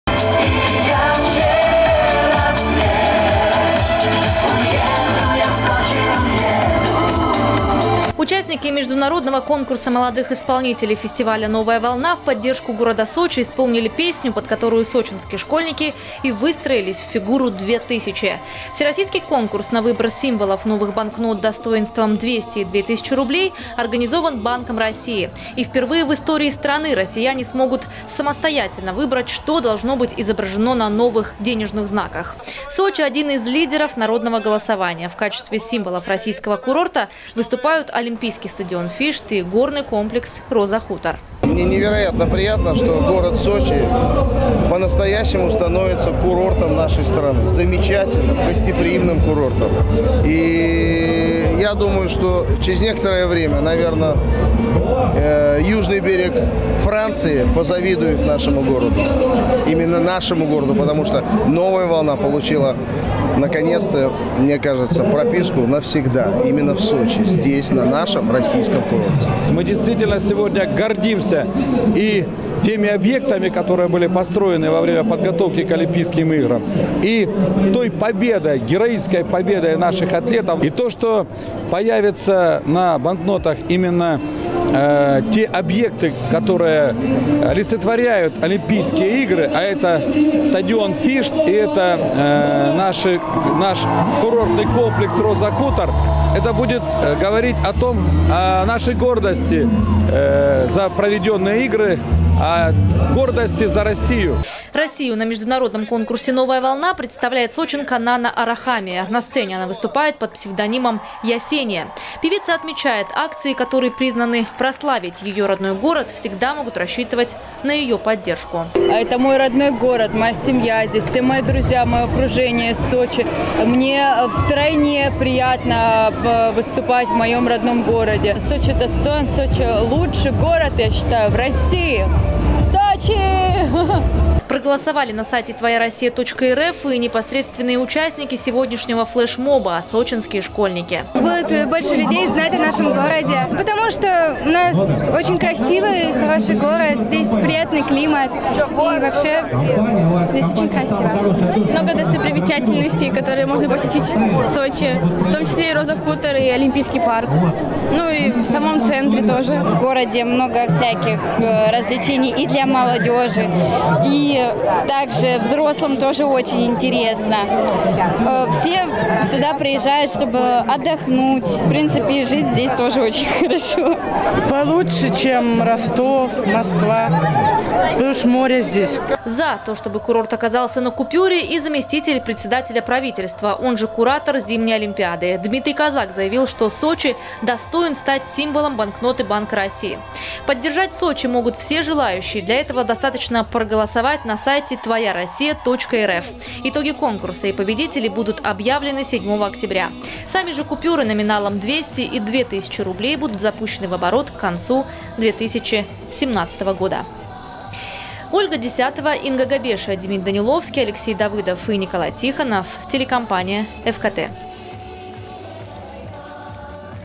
Флешмоб «Сочи-2000» поддержали звезды "Новой волны". Новости Эфкате Сочи
Две тысячи горожан собрались сегодня на сочинской площади Флага. Флешмобом «Сочи-2000» курорт выступил в поддержку проекта «Сочи на банкнотах». Сегодня стартовал заключительный третий этап всероссийского голосования. Присоединились к городской акции и конкурсанты «Новой волны».

Участники международного конкурса молодых исполнителей «Новая волна» в поддержку города Сочи исполнили песню, под которую сочинские школьники и выстроились в фигуру «2000». Всероссийский конкурс на выбор символов новых банкнот достоинством 200 и 2000 рублей организован Банком России. И впервые в истории страны россияне смогут самостоятельно выбрать, что должно быть изображено на новых денежных знаках. Сочи — один из лидеров народного голосования. В качестве символов российского курорта выступают олимпийский стадион «Фишт» и горный комплекс «Роза Хутор».

Сочи. Новости.
Телеканал Эфкате.